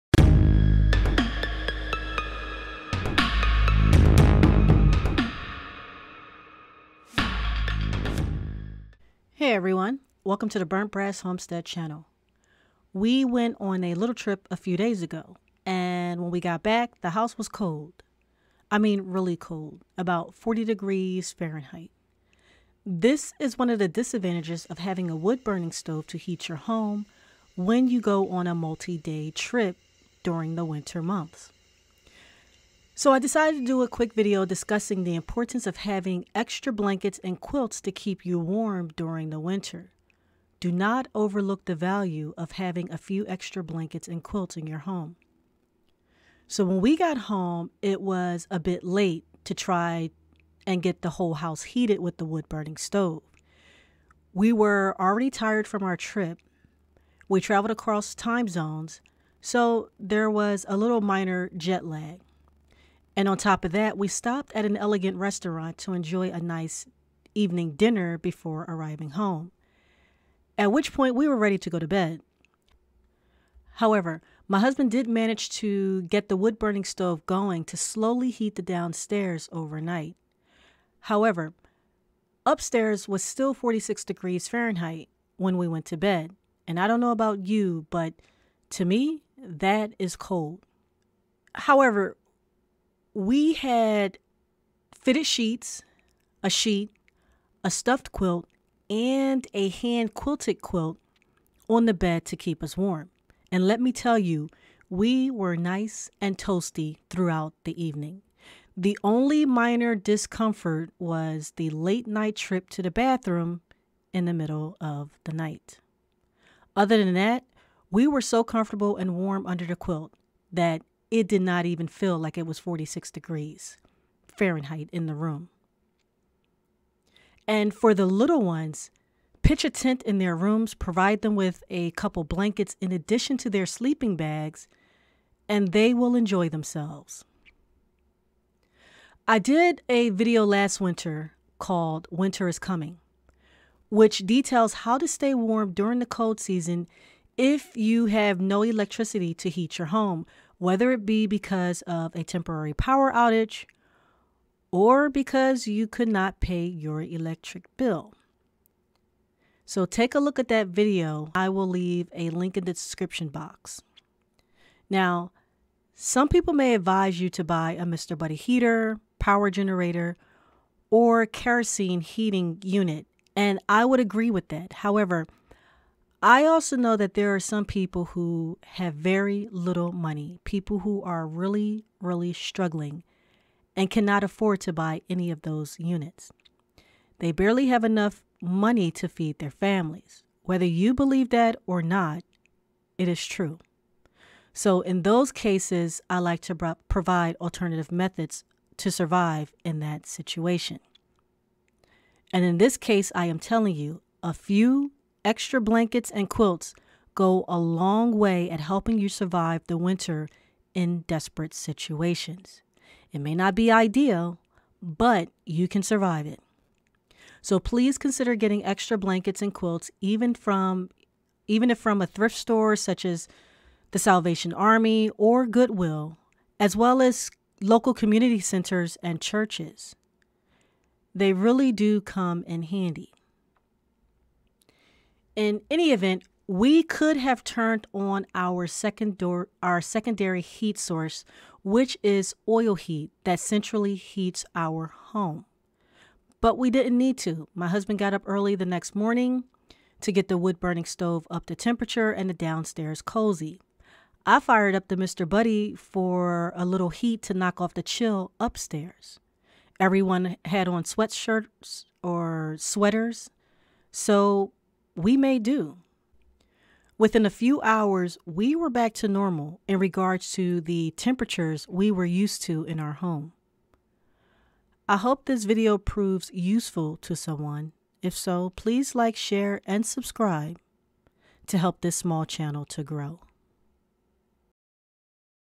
No Heat: Surviving In Home That's 40-Degrees Fahrenheit (Blankets Only)
Winter is almost over, but I wanted to do a video to show just how valuable blankets are during the cold season. We came back to a cold house after a short multi-day trip. But we made ourselves comfortable thanks to our extra blankets.